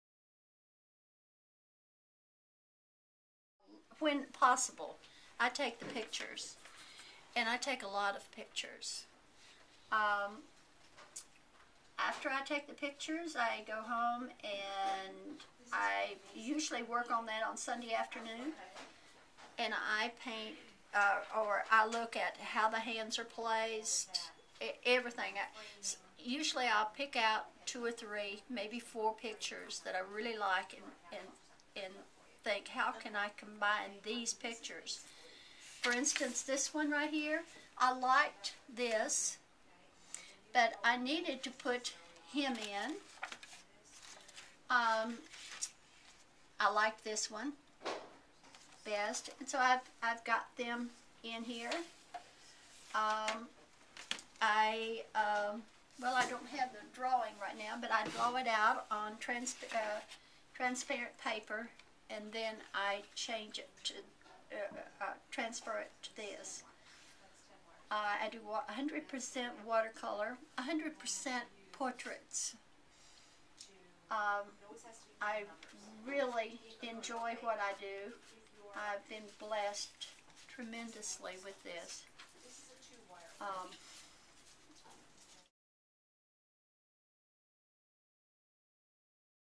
Inner Banks Artisans Center, Jerry Bradley talks about her art
Inner Banks is a dream that you are fulfilling. When I began working in metal I only imagined that my work would be in a gallery someday...someday is here and my dream is fulfilled. Many thanks to you for all the hard work and thought, and for allowing me to be a part of all the excitement. You make me feel so included.